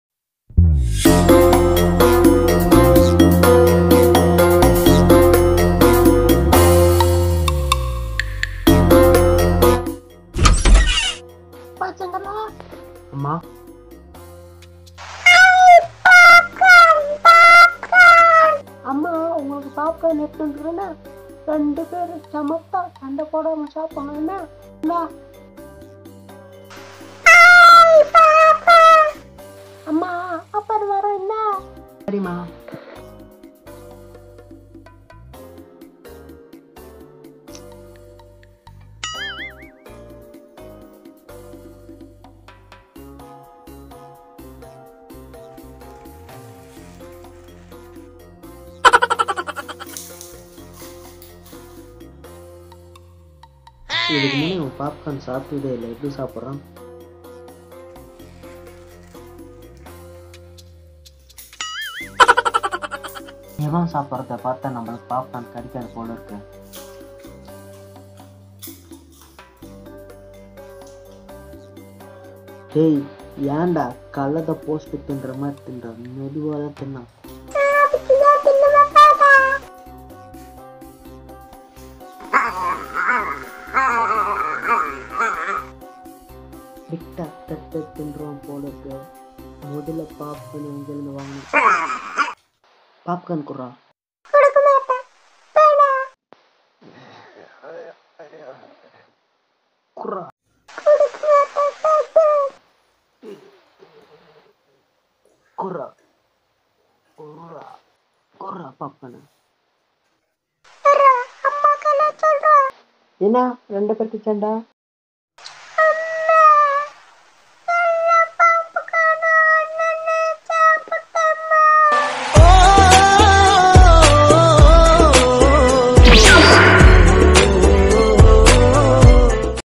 popcorn தம்பி
popcorn தம்பி# இந்தப் பதிவில் மூன்று வேடங்களில் அண்ணன் தம்பி சண்டை சின்னதான காமெடி முயற்சி செய்யப்பட்டுள்ளது sivasamy 94#